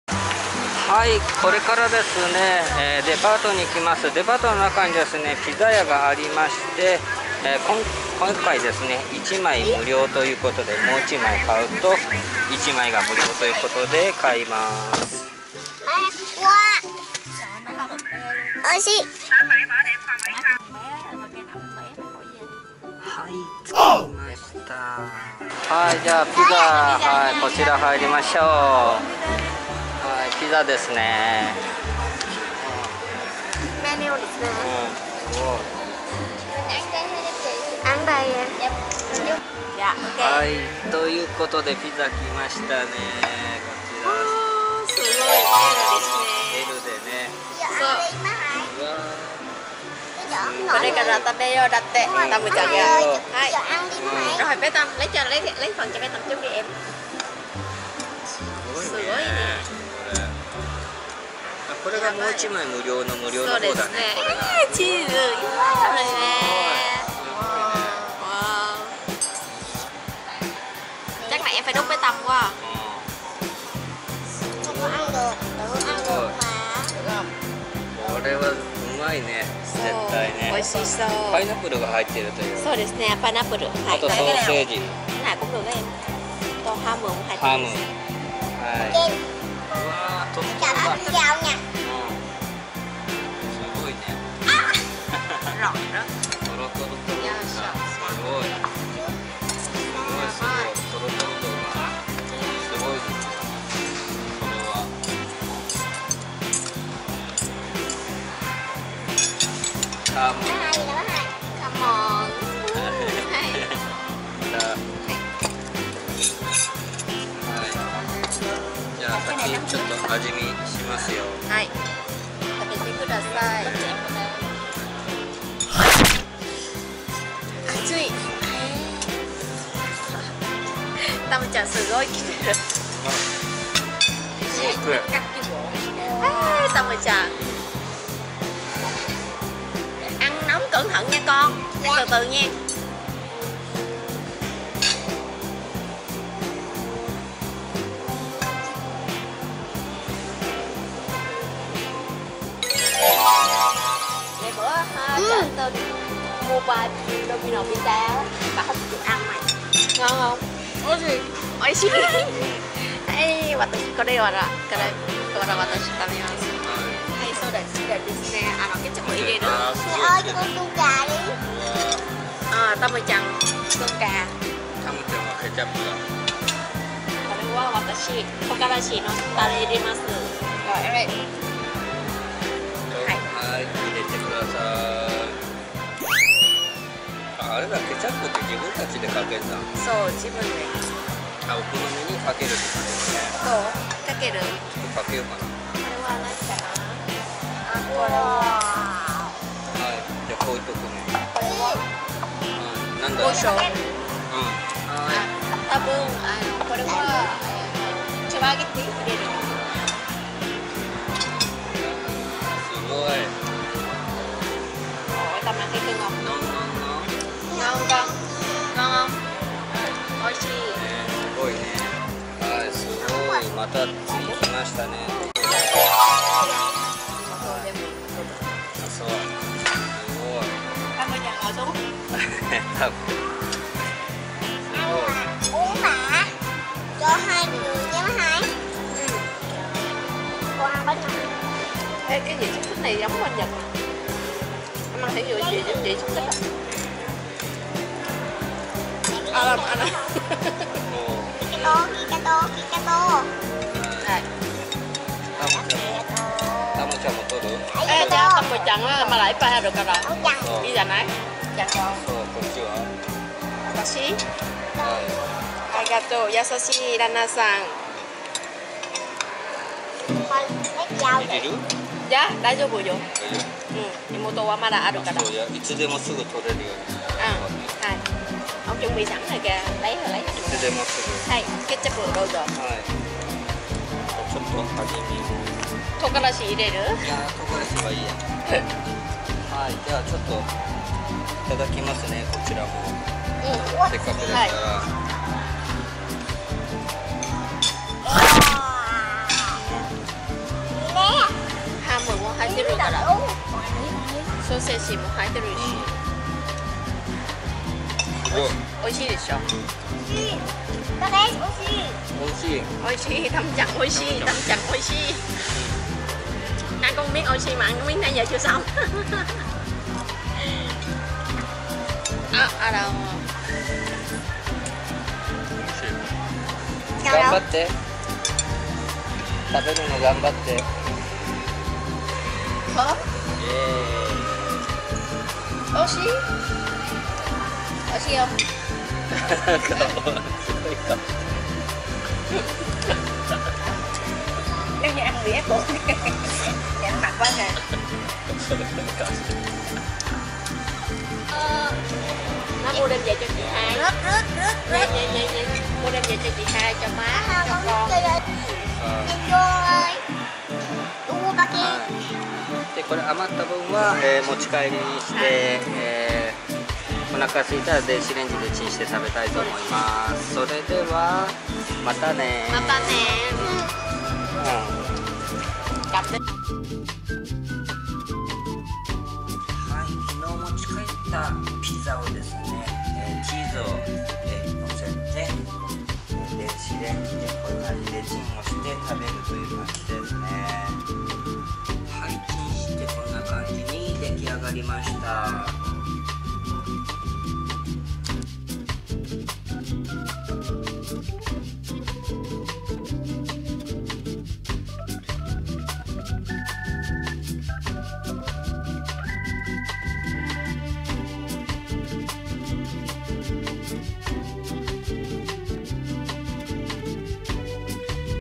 ベトナム国際結婚生活 #311🇻🇳日越夫婦と妹とタムちゃんとピザを食べに行ってみた！The Pizza Company
ベトナムの地方都市で生活している日本人とベトナム人の国際カップルです。デパートの中にあるThe Pizza Companyのお店で家族でピザを食べに行きました。ベトナムのピザは日本のピザと同じようにおいしかったですね。また機会がありましたら、みんなで食べに行きたいと思いましたね。

【 他のYouTubeチャンネルはこちら 】

◆本動画内の音楽については、STAR MUSICの許可を頂いております。